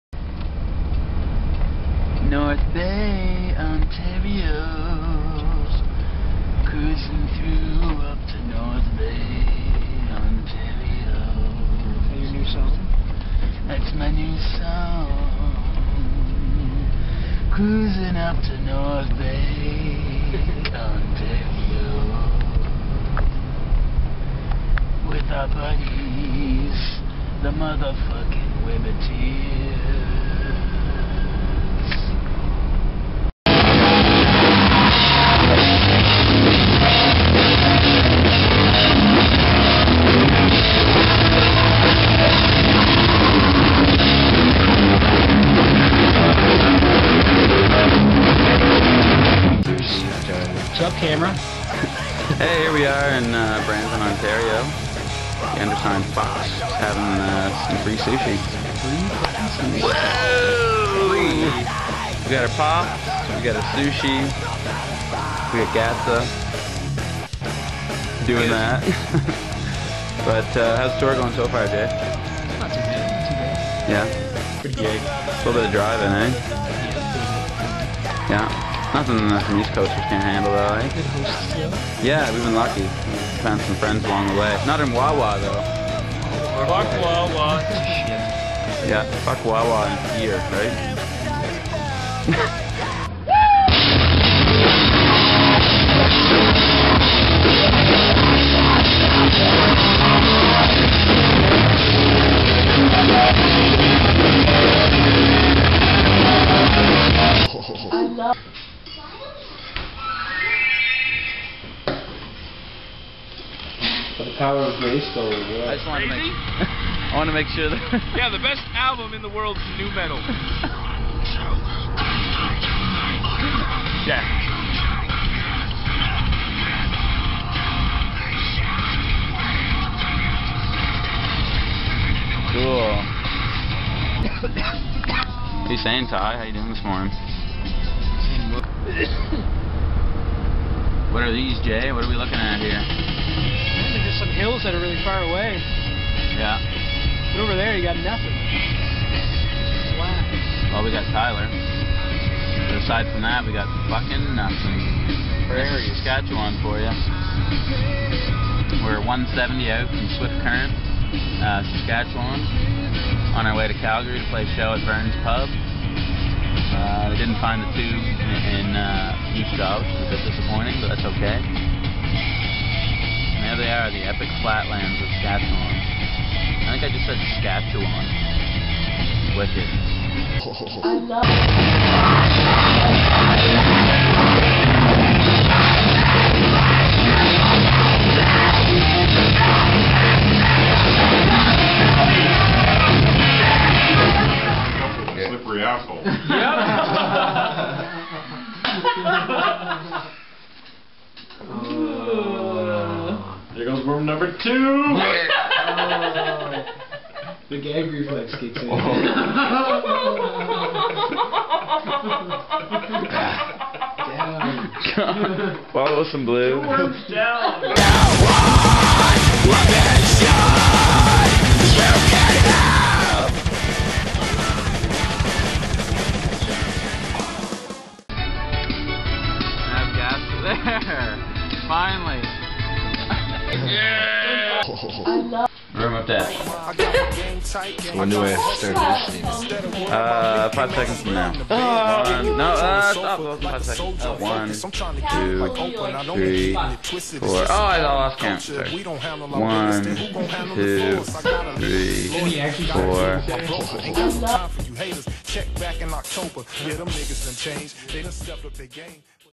We, The Undersigned - Whistle Goes WOOOOO!! video blog 1 of 3
first of 3 blogs of our cross Canada tour from march 25th till May 4th